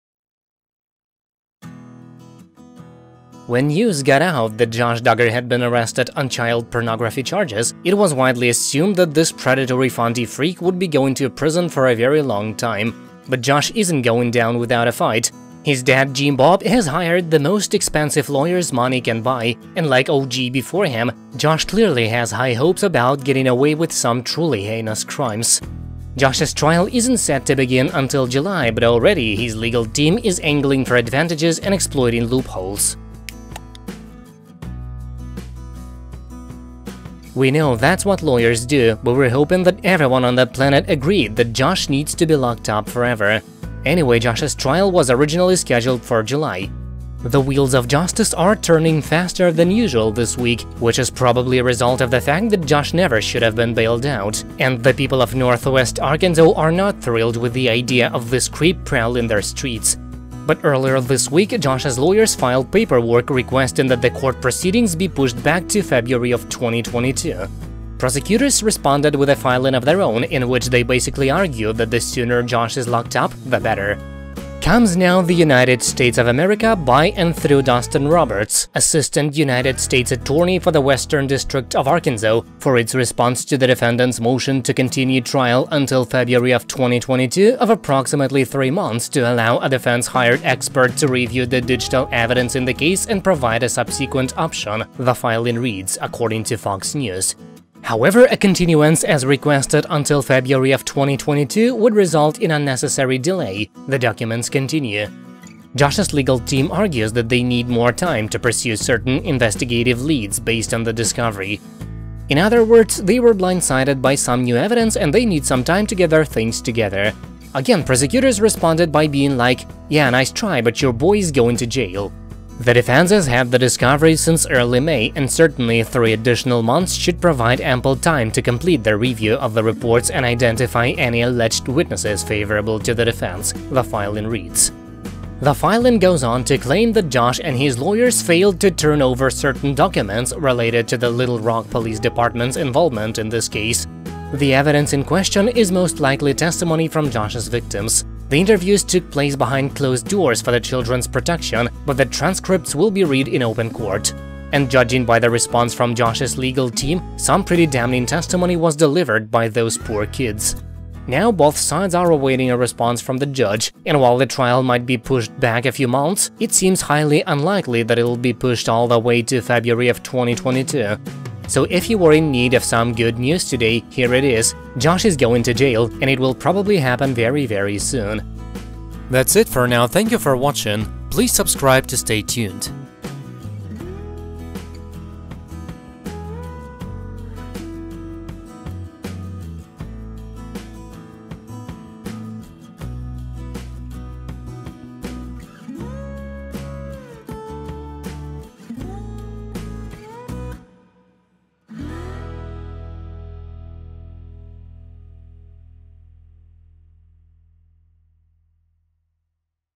Judge to Josh Duggar - Buddy, You're Going to Jail Real Soon! - Counting On TLC
When news got out that Josh Duggar had been arrested on child pornography charges, it was widely assumed that this predatory fundie freak would be going to prison for a very long time.    But Josh isn't going down without a fight.    His dad, Jim Bob, has hired the most expensive lawyers money can buy, and like O.J. before him, Josh clearly has high hopes about getting away with some truly heinous crimes.  Josh's trial isn't set to begin until July, but already, his legal team his legal team is angling for advantages and exploiting loopholes.  We know that's what lawyers do, but we were hoping that everyone on the planet agreed that Josh needs to be locked up forever.    Anyway, Josh's trial was originally scheduled for July.  The wheels of justice are turning faster than usual this week, which is probably a result of the fact that Josh never should have been bailed out, and the people of Northwest Arkansas are not thrilled with the idea of this creep prowling their streets.    But earlier this week, Josh's lawyers filed paperwork requesting that the court proceedings be pushed back "